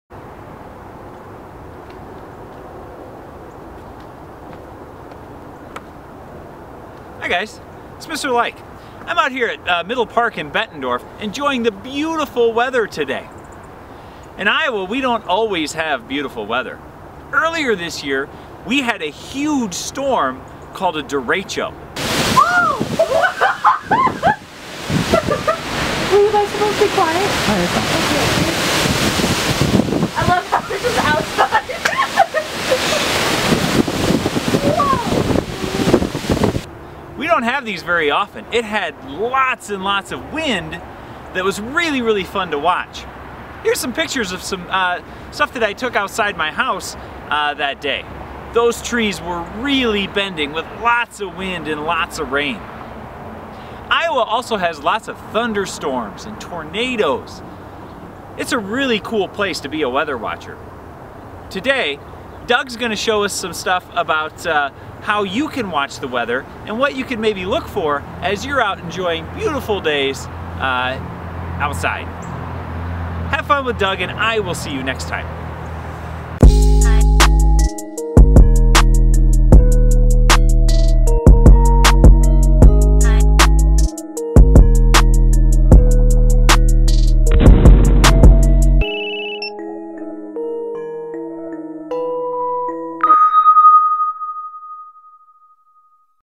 Kindergarten Weather: Lesson 1
Description:
NGSS Standards: ESS2-1, ESS2-2, ESS3-1, ESS3-2, ESS3-3, PS3-1, PS3-2
These lessons are made to be watched in order. Each grade level unit begins with Episode 1 and contains student videos, student worksheets, and parent videos.

Student Instructions:
1.Open the slide deck located here to follow along:

2. Go through the slide deck, filling in any boxes in blue.
3.Contact your teacher with any questions.
4.Turn in the worksheet to your teacher for feedback or credit.

Credits:
Background Music and Sound Effects:
Various Images: Google Images